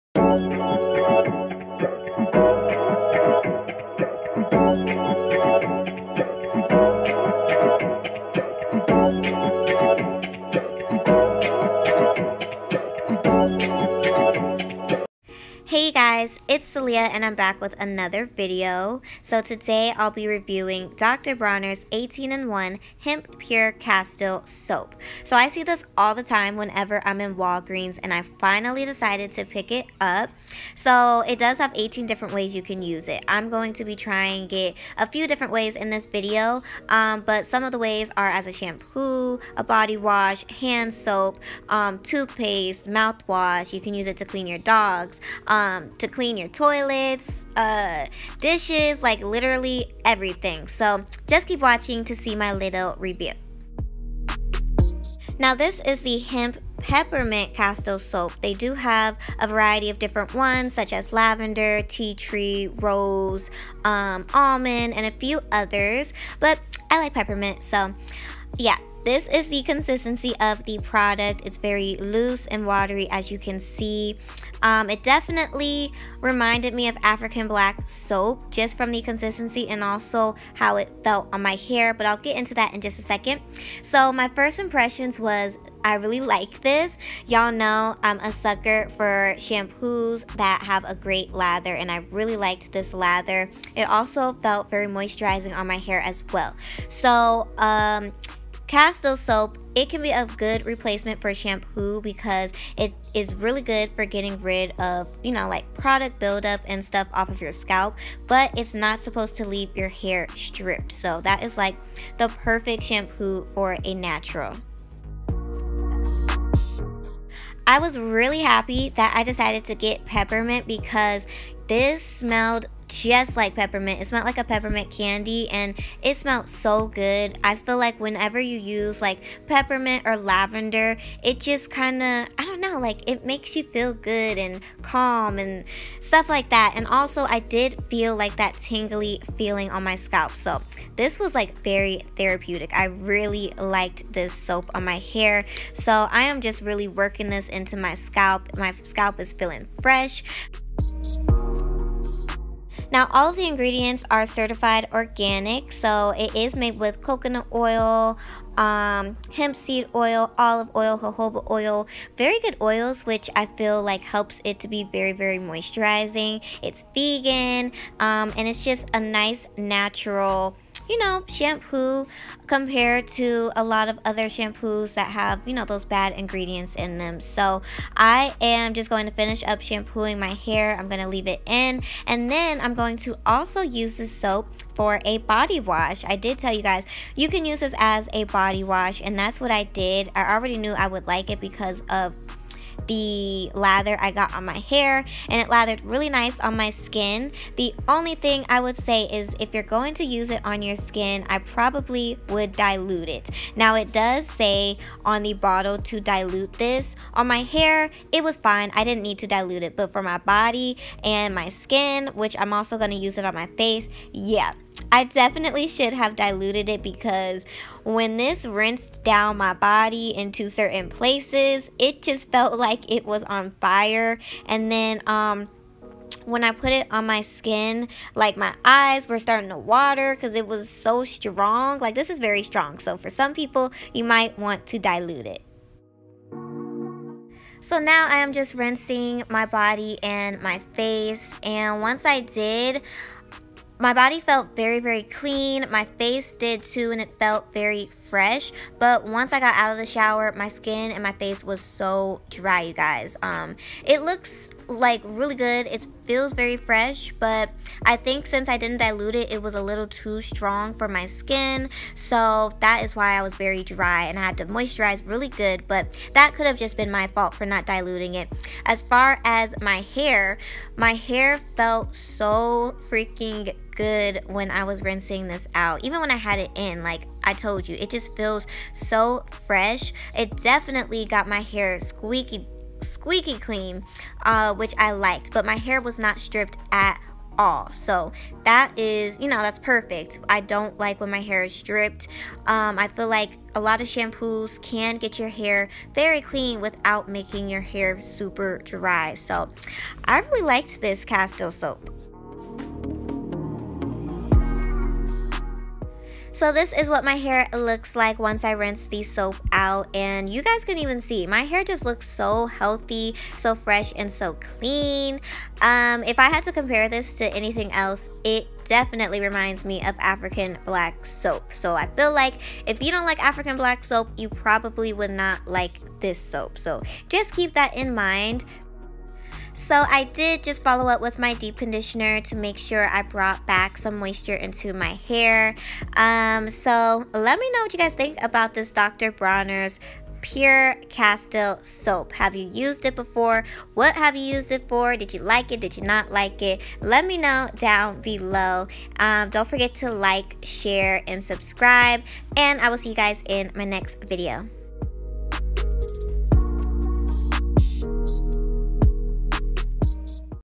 Dr. Bronners 18 in 1 PURE CASTILE Soap | A Shampoo, Bodywash, Facewash and More?
Hey you guys. In today's video, I wanted to try this soap I always see in stores, but never try. I am trying Dr. Bronners 18 in 1 Hemp Castile Soap. This soap can be used in 18 different ways, from washing your hair and body, cleaning your home, brushing your teeth and more. XOXO.

Dr. Bronners Peppermint Castile Soap

Caaliyah Greer